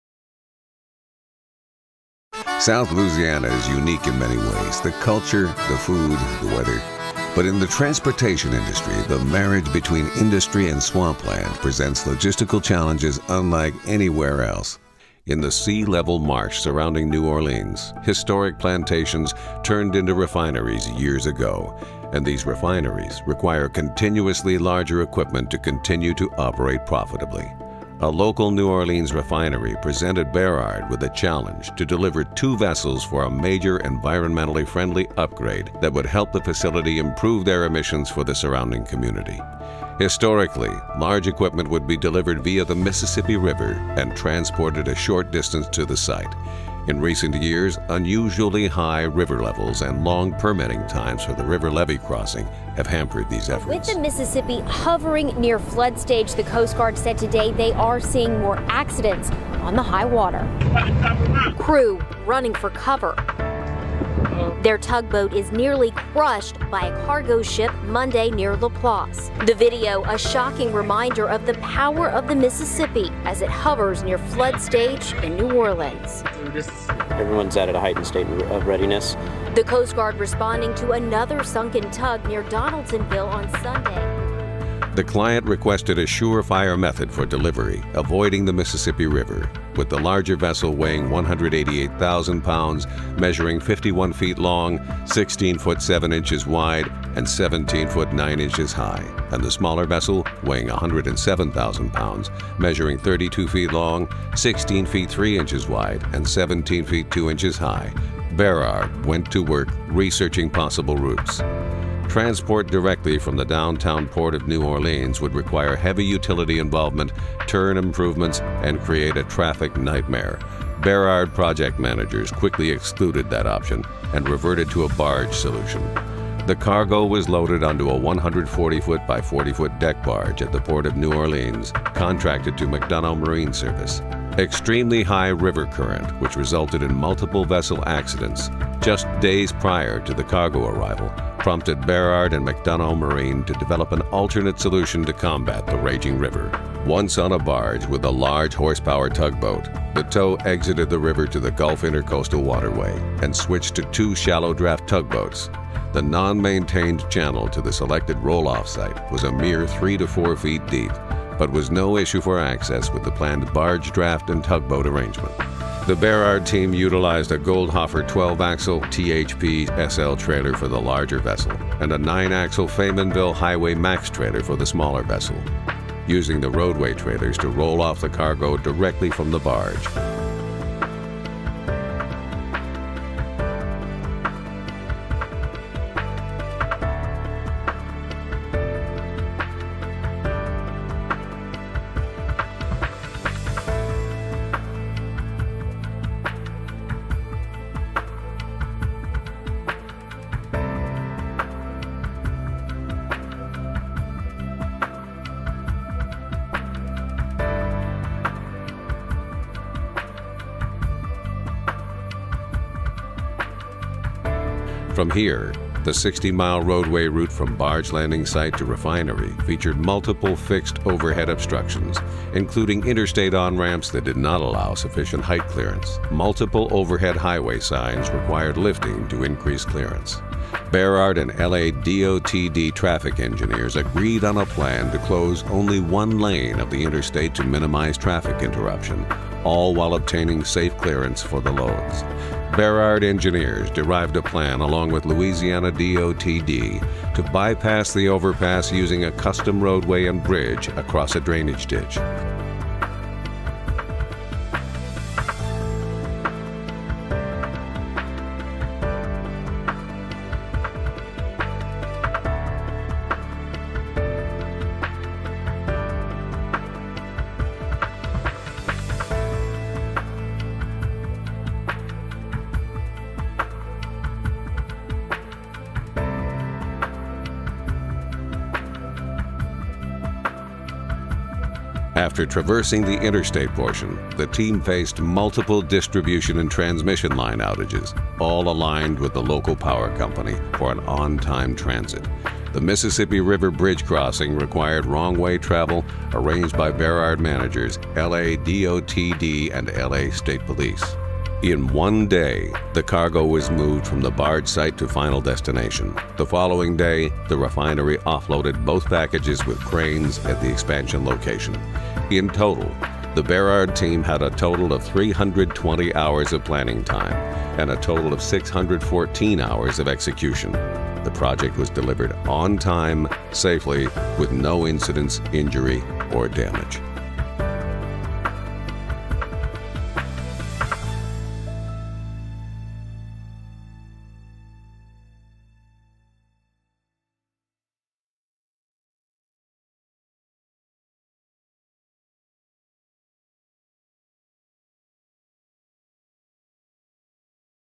Berard Vessel Transports
A local New Orleans refinery presented Berard with a challenge to deliver two vessels for a major environmentally friendly upgrade that would help the facility improve their emissions for the surrounding community.

Historically, large equipment would be delivered via the Mississippi River and transported a short distance to the site. In recent years, usually high river levels and long permitting times for the river levee crossing have hampered these efforts.

The client requested a sure-fire method for delivery avoiding the Mississippi River. With the larger vessel weighing 188,000 lbs., measuring 51’ long, 16’ 7” wide, and 17’ 9’ high, and the smaller vessel weighing 107,000 lbs, measuring 32’ long, 16’ 3” wide, and 17’ 2” high, Berard went to work researching possible routes.
Transport directly from the downtown Port of New Orleans would require heavy utility involvement, turn improvements, and create a traffic nightmare. Berard project managers quickly excluded that option and reverted to a barge solution.

In one day, the cargo was moved from the barge site to final destination. The following day, the refinery offloaded both packages with cranes at the expansion location. In total, the Berard team had a total of  320 hours of planning time, and a total of 614 hours in execution. The project was delivered on time, safely with no incidents, injury, or damage.